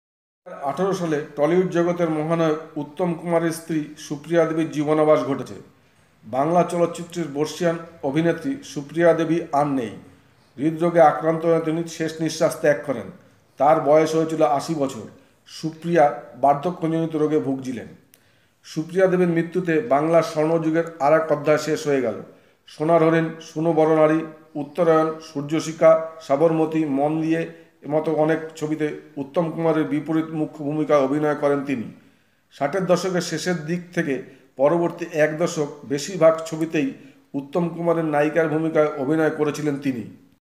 Exclusive: ২০১৮ সালে স্বর্গবাসী মহানায়কের স্ত্রী
২০১৮ সালে টলিউড জগতের মহানায়ক উত্তম কুমারের স্ত্রী সুপ্রিয়া দেবীর জীবনাবসান ঘটেছে।বাংলা চলচ্চিত্রের বর্ষীয়ান অভিনেত্রী সুপ্রিয়া দেবী আর নেই। হৃদরোগে আক্রান্ত হয়ে তিনি শেষনিশ্বাস ত্যাগ করেন। তাঁর বয়স হয়েছিল ৮৩ বছর। সুপ্রিয়া বার্ধক্যজনিত রোগে ভুগছিলেন।

সুপ্রিয়া দেবীর মৃত্যুতে বাংলার স্বর্ণযুগের আরেক অধ্যায় শেষ হয়ে গেল। সোনার হরিণ, শুন বরনারী, উত্তরায়ণ, সূর্যশিখা, সাবরমতী, মন নিয়ে-এর মতো অনেক ছবিতে উত্তম কুমারের বিপরীতে মুখ্য ভূমিকায় অভিনয় করেন তিনি। ষাটের দশকের শেষের দিক থেকে পরবর্তী এক দশক বেশির ভাগ ছবিতেই উত্তম কুমারের নায়িকার ভূমিকায় অভিনয় করেছিলেন তিনি।